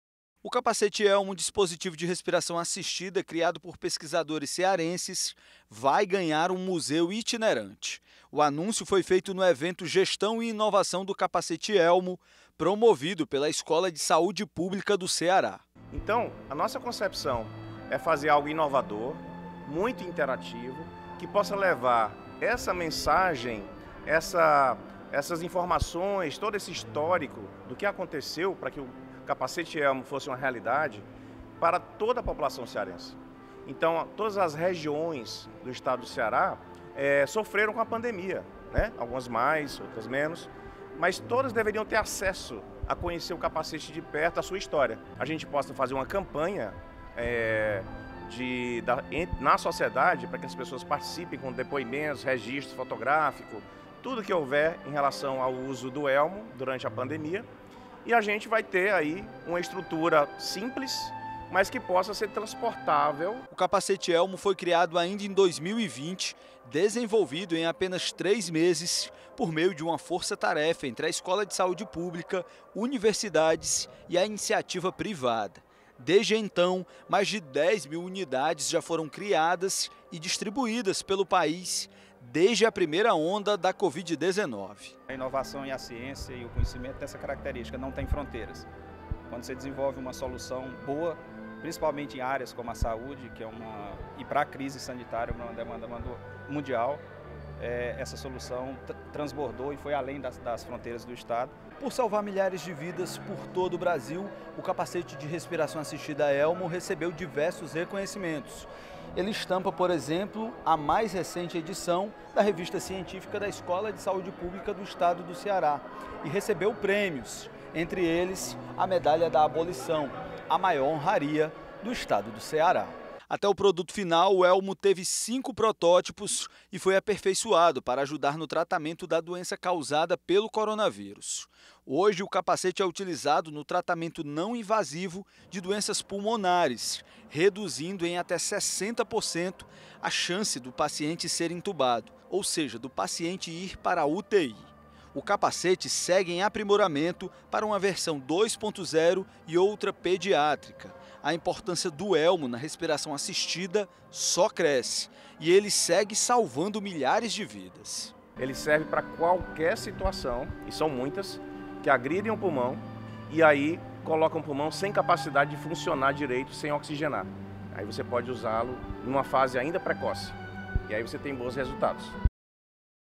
Origem do Capacete Elmo será contada através de projeto da Escola de Saúde Pública do Ceará
JornaldaTVC Notícias | 1ª edição. A história do Capacete Elmo deve ser levada e contada por todo estado através de um projeto inovador da Escola de Saúde Pública do Ceará. Confira mais detalhes na reportagem.